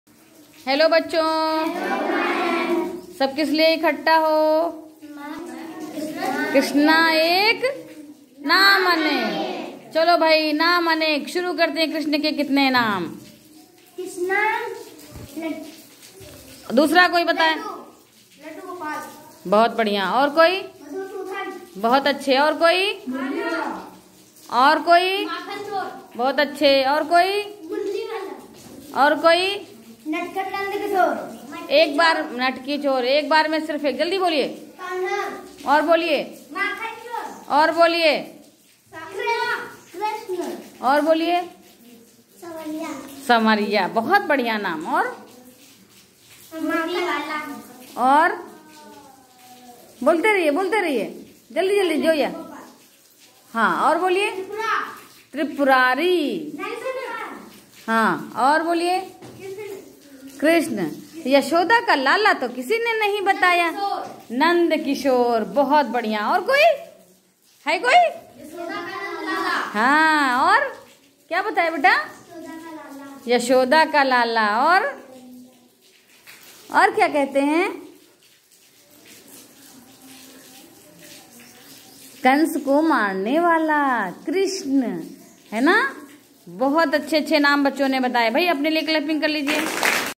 Krishna Ek Naam Anek | Primary School | Kanchan Lata
Happy Janmashtami | Kaaliya Naag | School Play | Primary School | Kanchan Lata